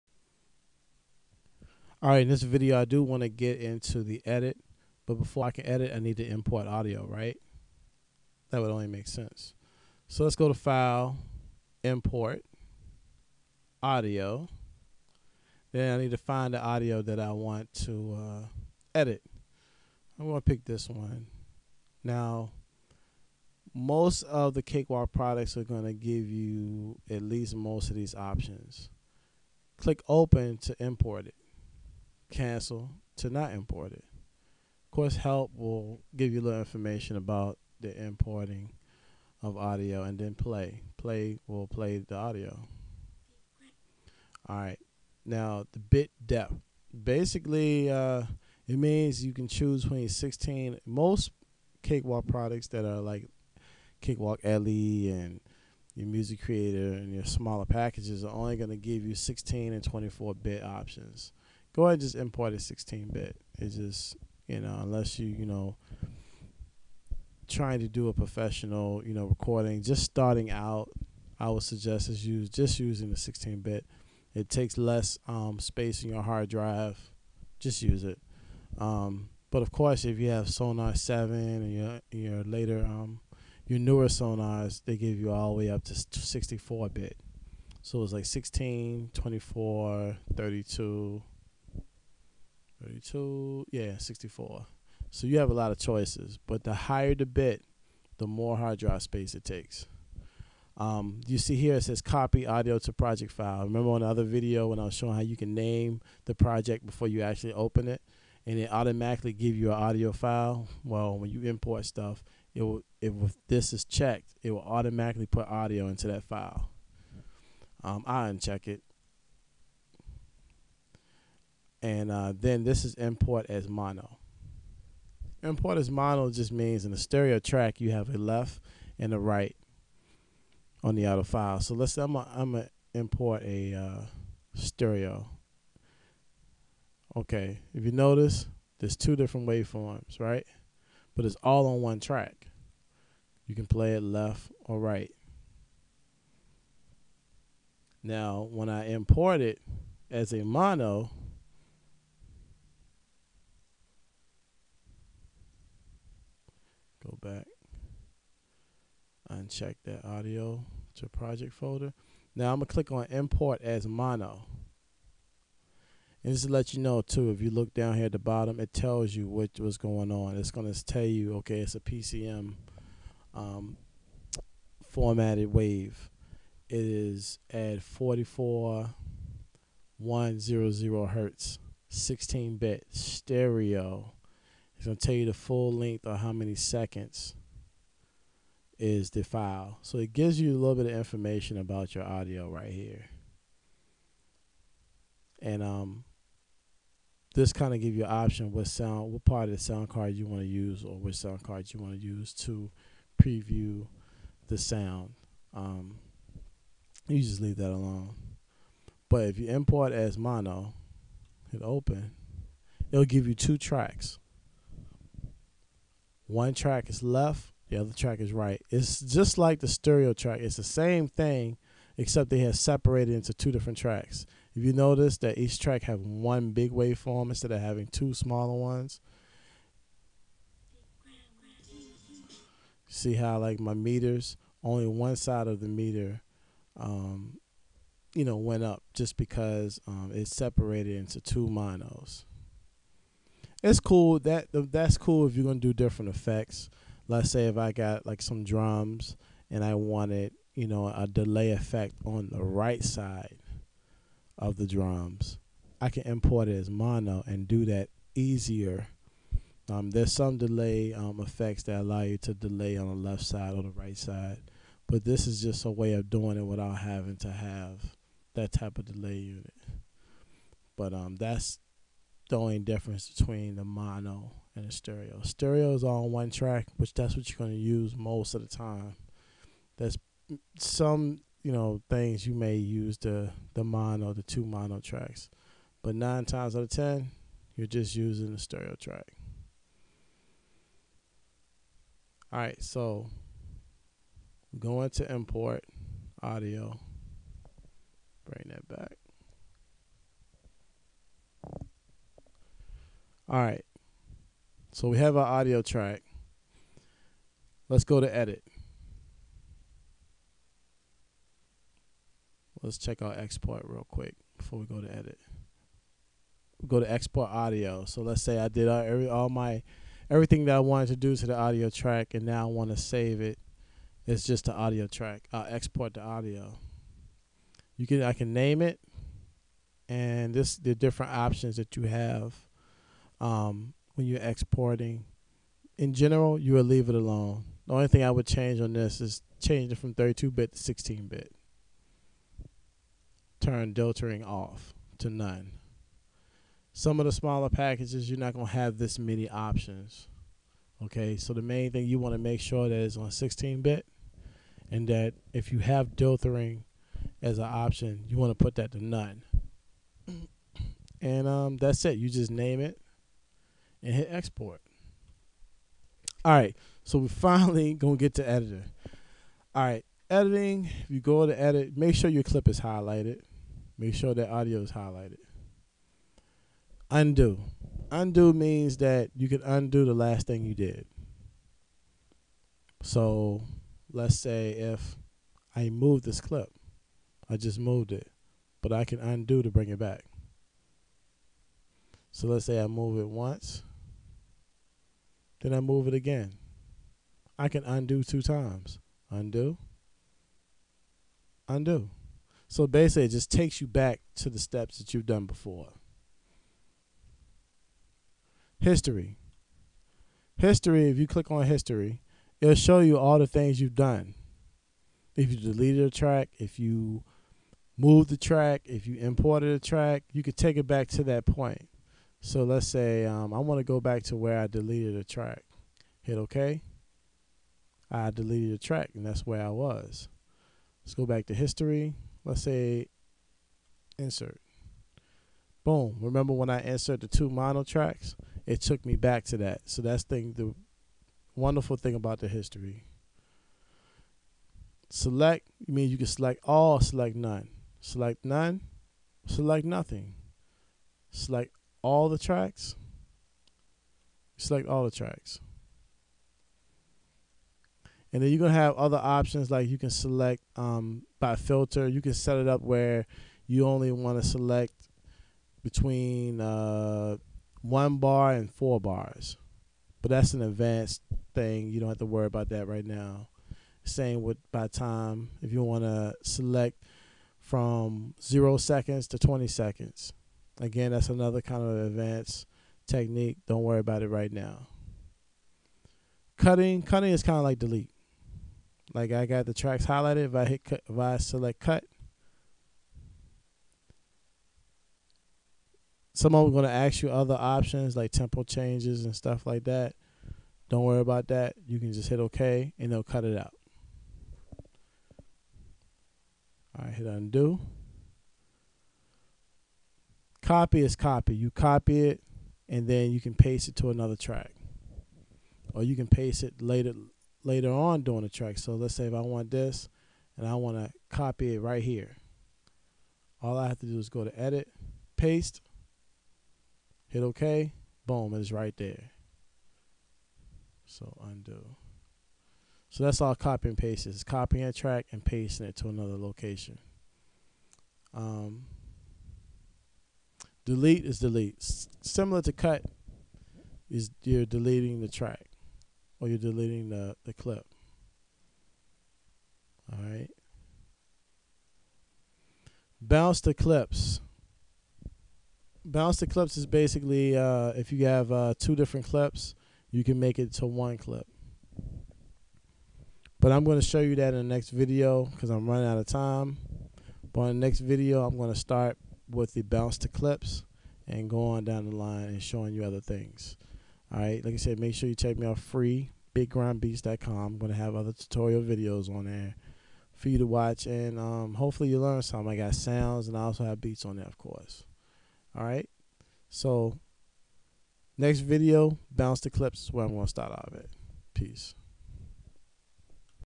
Cakewalk Sonar beginer tips by Big Grime Vol 3
This is where I go into the edit section. I don't really get to any advance stuff but, you will understand what this section does.